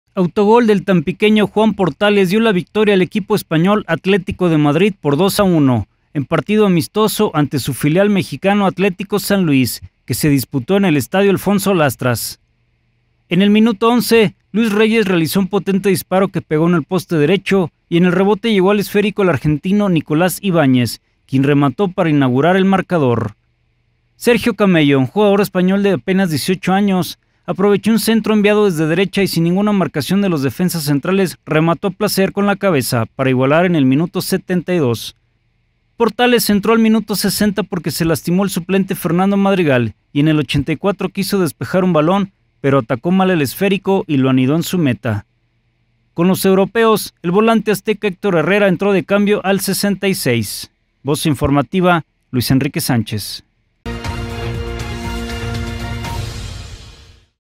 Colchoneros certeros
En amistoso en tierras potosinas entre Atléticos, el Madrid venció 2-1 al San Luis; Herrera entró al 66